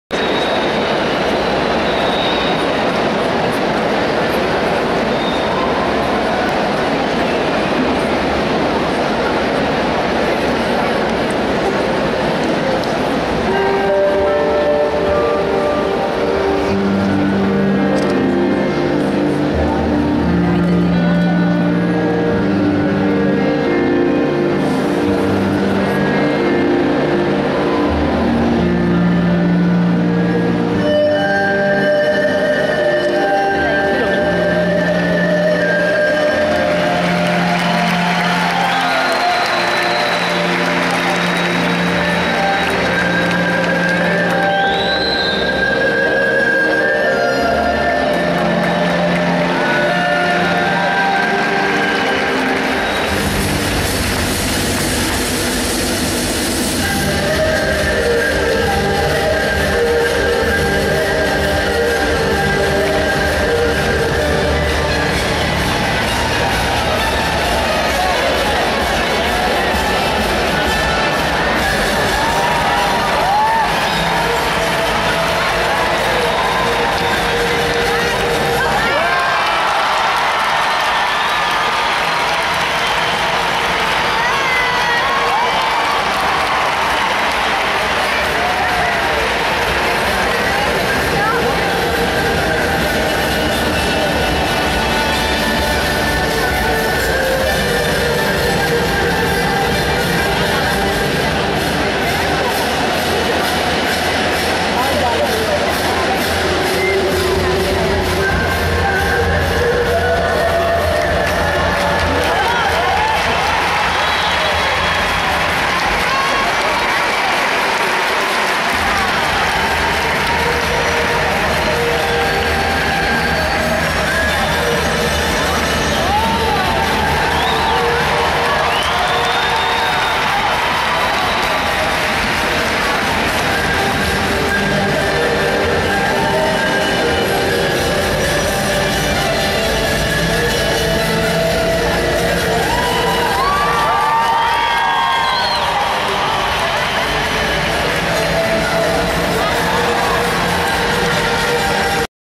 CHINA - World Games Cali 2013 Men group final (no ending)
CHINA - Men group final

ZHOU Yi / TANG Jian / WU Yeqiuyin / WANG Lei

'13 World Games in Cali qualifications / Jeux mondiaux

29.865 (Diff: 12.565 # value 401 / Art: 8.6 / Ex: 8.7)
Rank : 1st